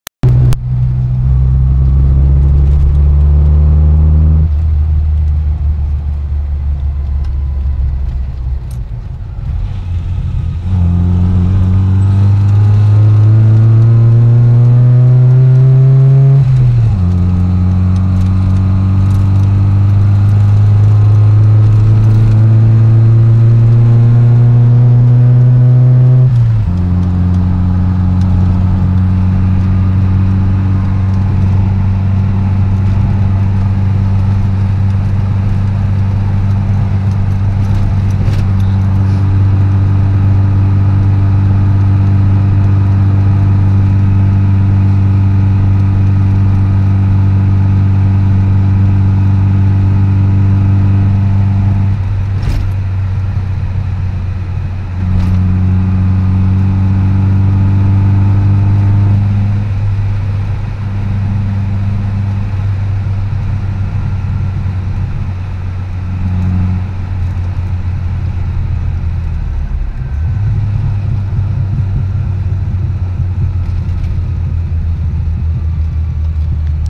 Drive in 998 1985 Mini Mayfair
A short slow drive in my  998 1985 Mini Mayfair. Full stainless exhaust system, K&N cone air filter with K&N sub stack.